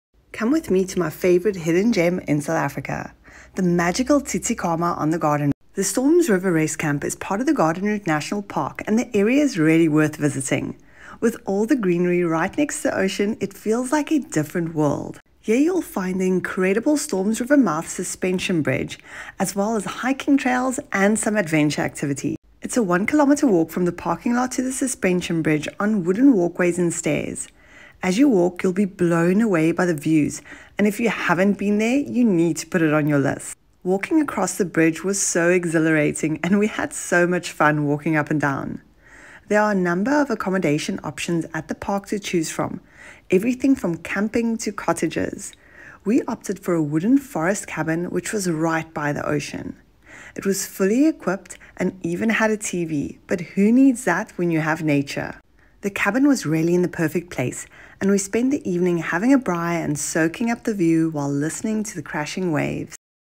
I VISITED A HIDDEN GEM IN SOUTH AFRICA! | TSITSIKAMMA
Discover the enchanting beauty of South Africa's hidden gem, the Storms River Mouth Rest Camp in Tsitsikamma! Nestled amidst breathtaking coastal landscapes, this tranquil haven offers an unforgettable retreat for nature lovers and adventure seekers alike. Join us as we explore the wonders of this pristine paradise, from its lush forests to its crystal-clear waters. Get ready to embark on a journey of discovery and serenity unlike any other. Don't miss out – watch now and start planning your next adventure!